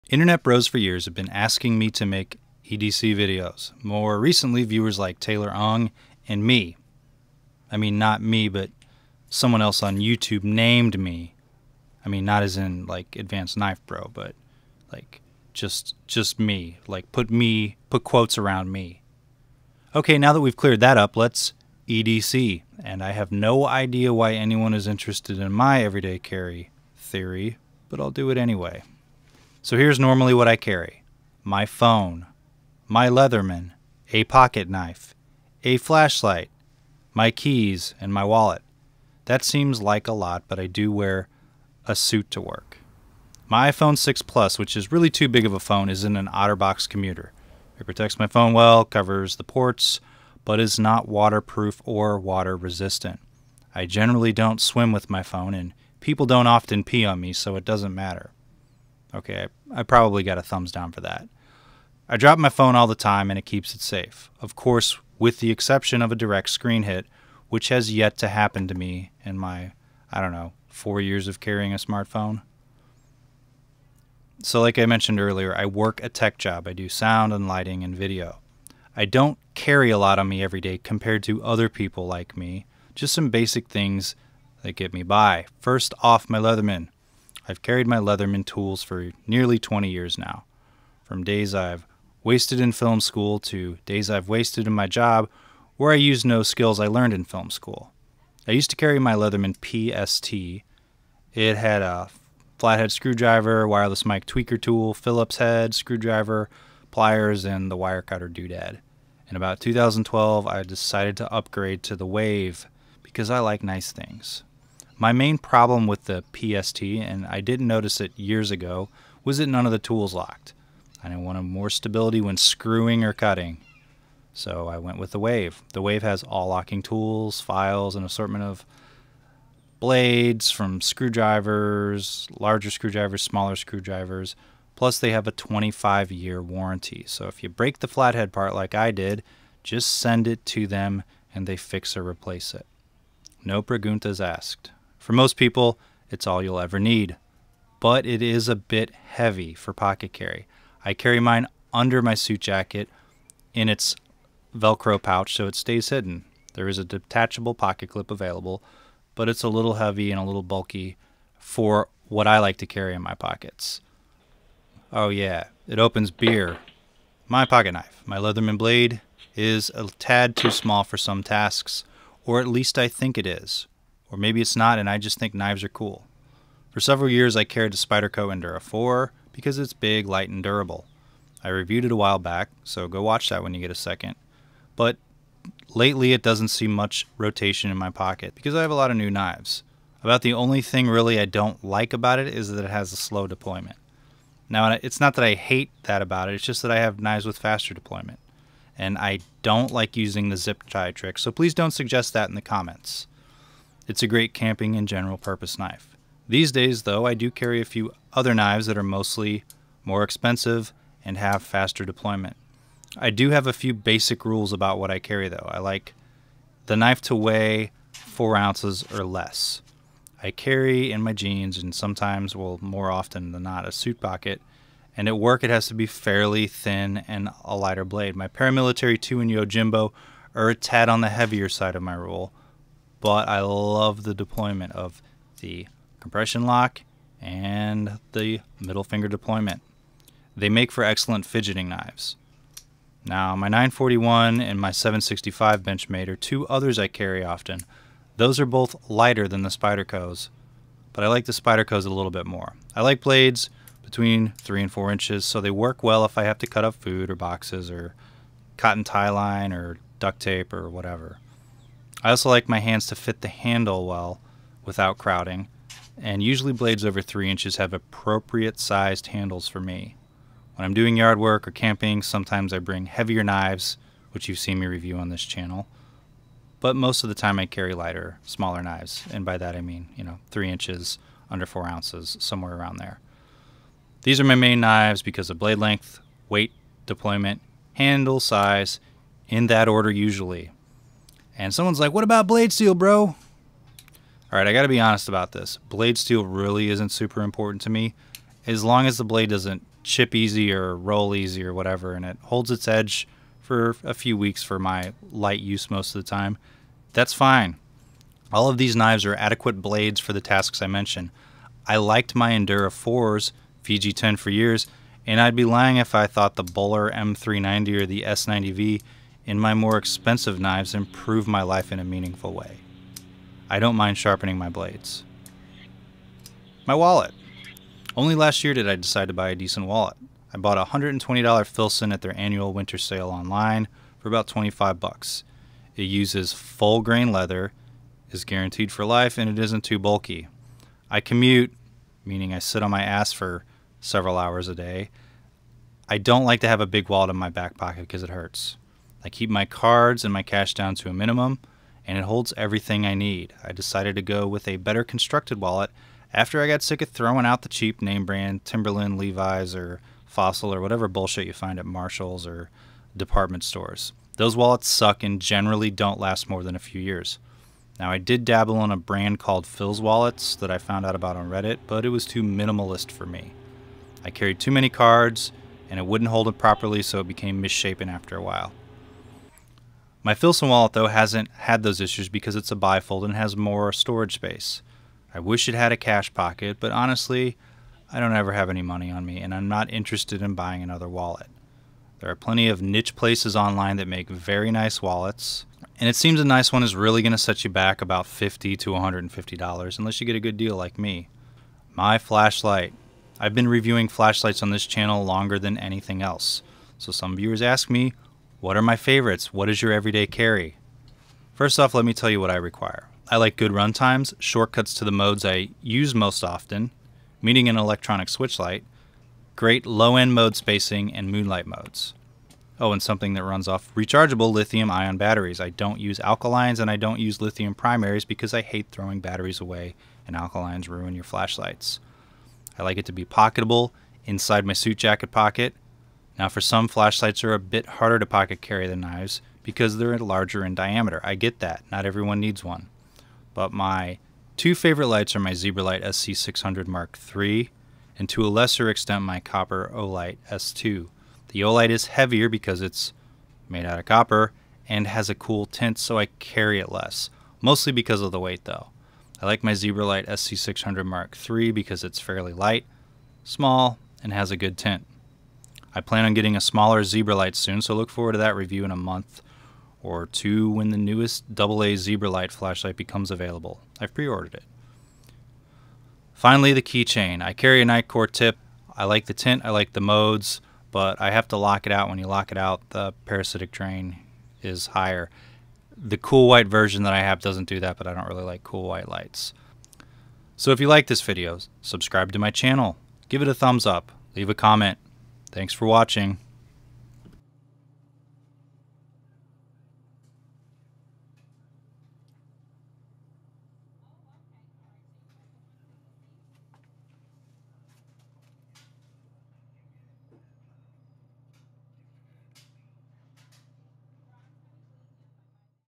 EDC Pocket Dump or The Things I Every Day Carry
This viewer request video has been coming for some time.  In this video I go over some basics of why I carry a multitool, a flashlight, and pocket knife.  I talk about my preferences for what I carry.  In this video you'll see the Leatherman Wave, the Leatherman PST, the iPhone 6 Plus, an Otterbox Commuter, a Filson Wallet, a Zebralight SC600w Mk III, a Benchmade 940-1, a wallet from Phils Wallets, a Nitecore TIP, the Benchmade 765, the Spyderco Endura 4, the Olight S2 copper, The Spyderco Yojimbo 2, the Spyderco Para Military 2, and more.  I have reviewed most of these EDC items, so check out my reviews for those.